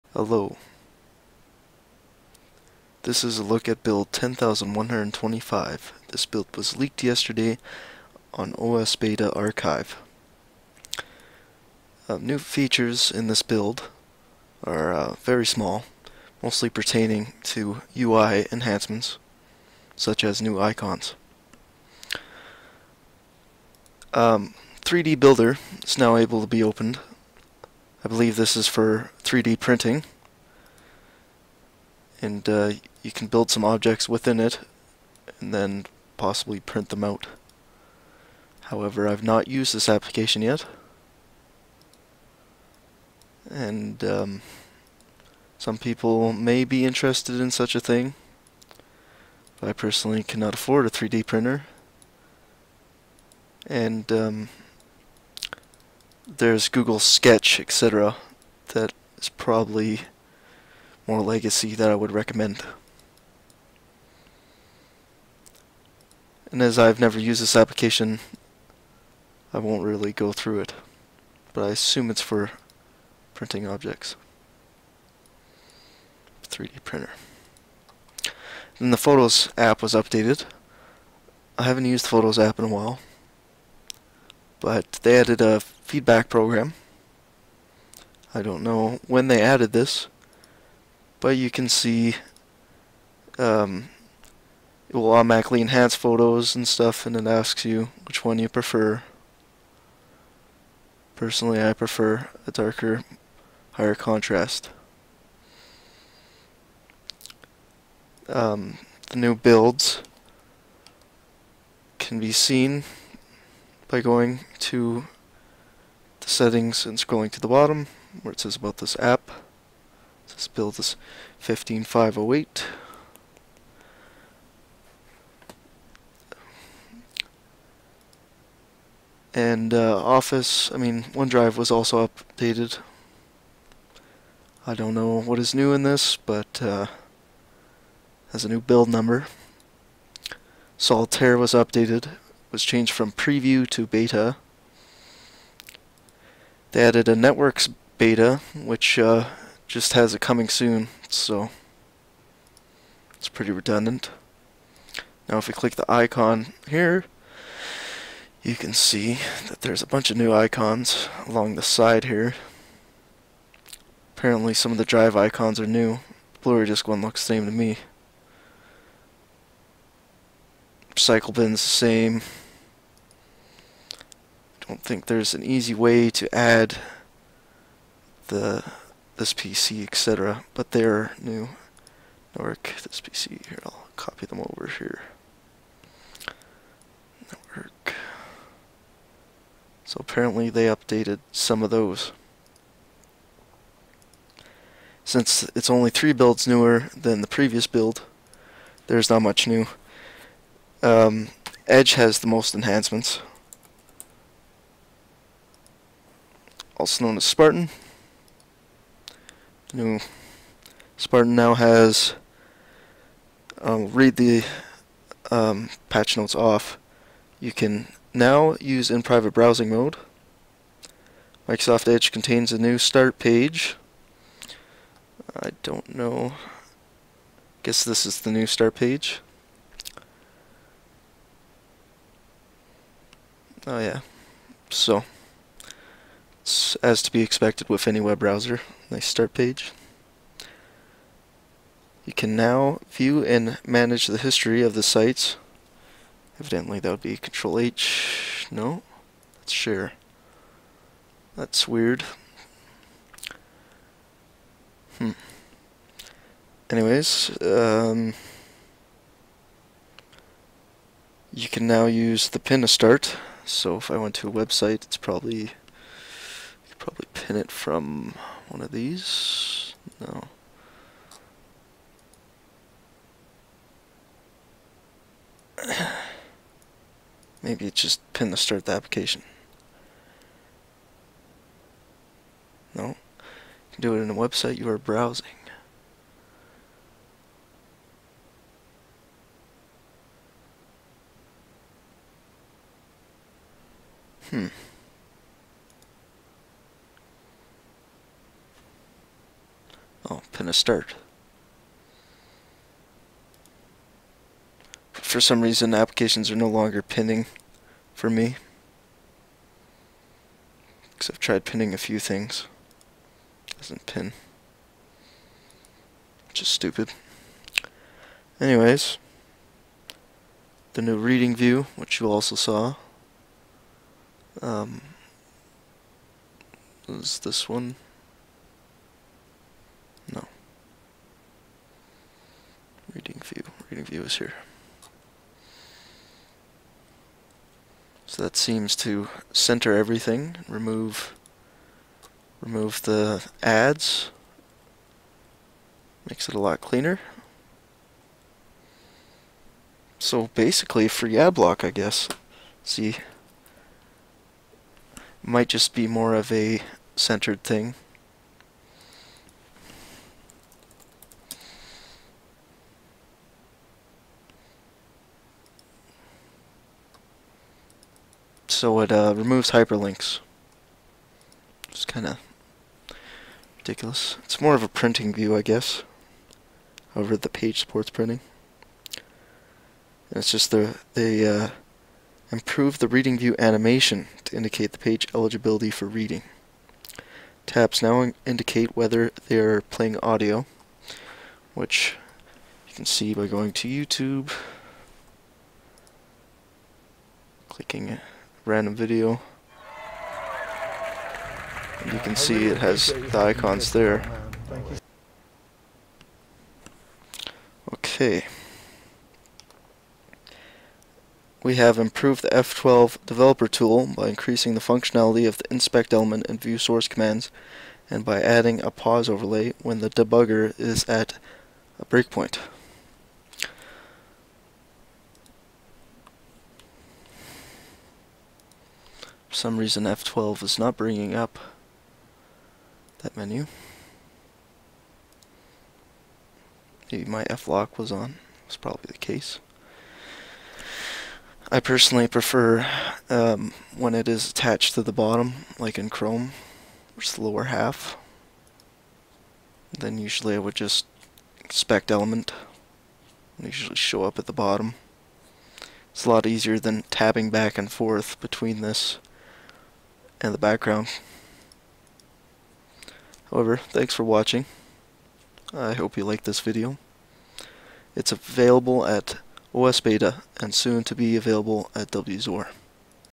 Windows 10 10125 First Look [Leaked] Download
Quick look at a leaked preview build of windows 10, Build 10125.
3D Builder Now Works, Spartan[Edge] has some new enhancements, Solitaire changed from preview to beta, new icons, Network beta app[place holder], new build of onedrive, photos and 'alarm and clock' however 'alarm and clock' crashes upon opening
Mouse is off center because I'm running a custom resolution, and obs is down sampling.
warning upon install the initial selecting who to login in as during setup hangs for up to 10 minutes.

If you ever buy on amazon please add &tag=chaython-20 at the end of the url